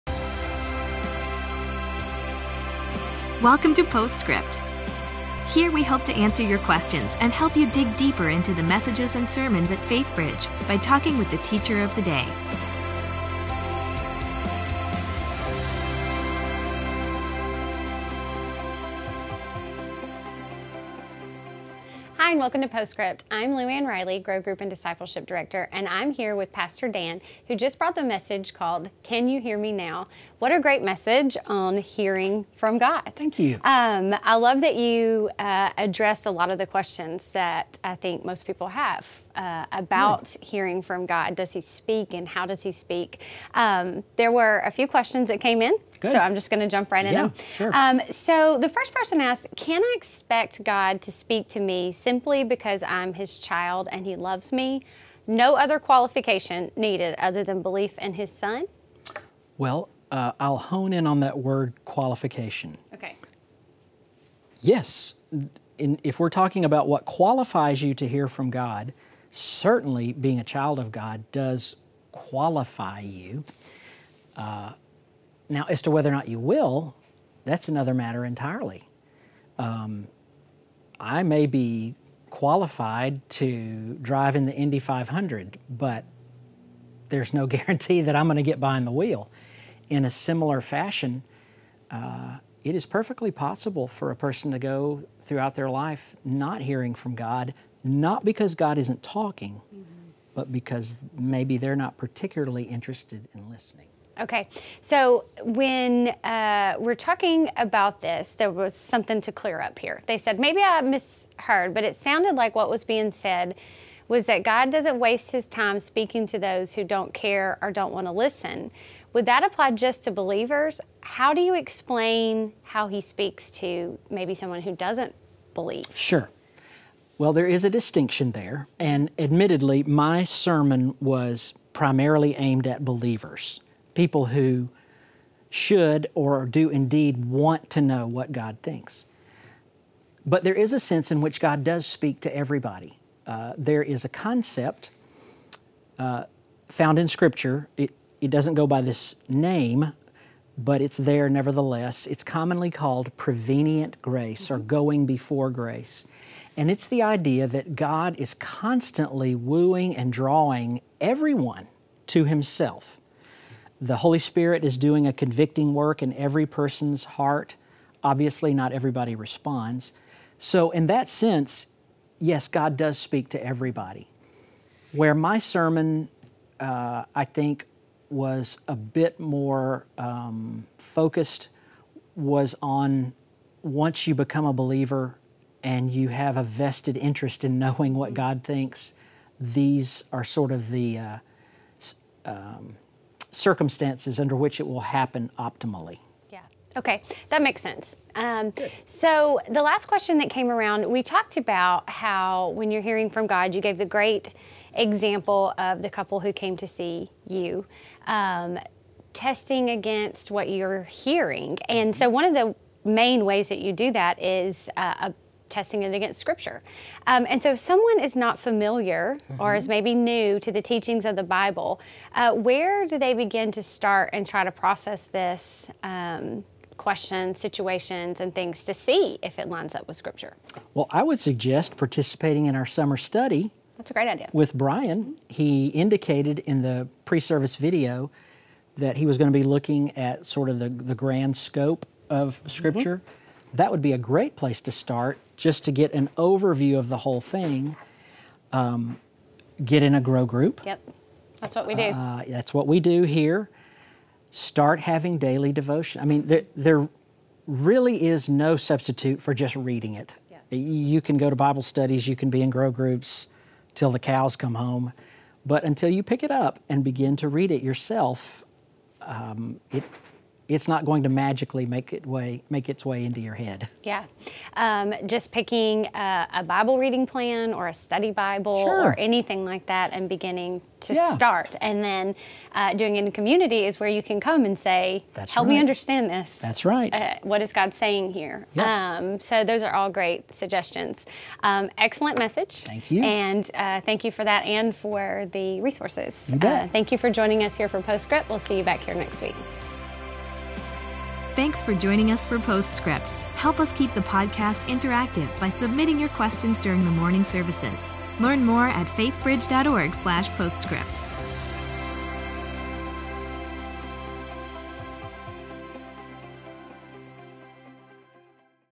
Postscript: Can You Hear Me Now?
Luann Riley hosts Pastor Dan Slagle, answering your questions about things you can do to hear from God.